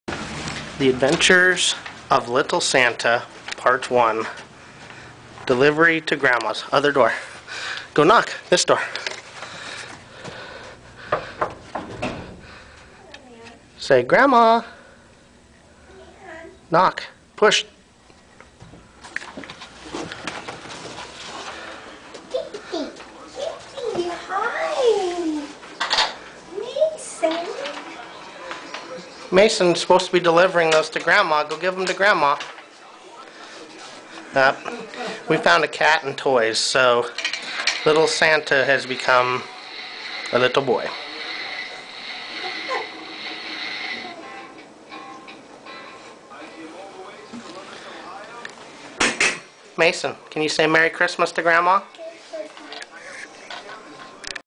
Mason playing Santa
Mason delivering a picture he made for hhis G'Ma Judy!  He decides the cat is more fun!!